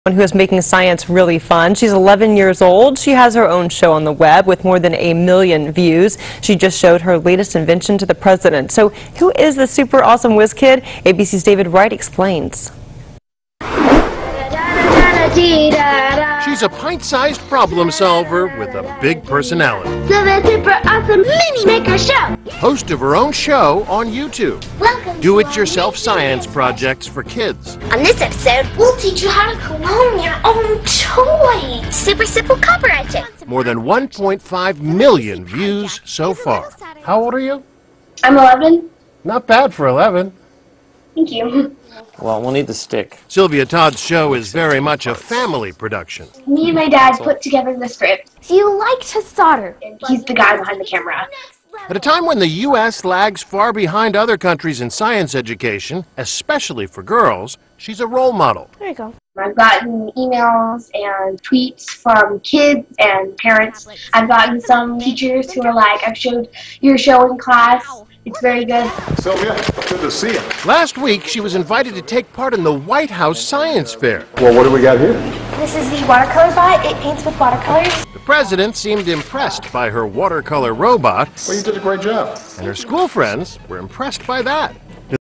11-Old-Girl Loves Science
An 11-year-old girl is hosting a show on Youtube about do it yourself science experiments.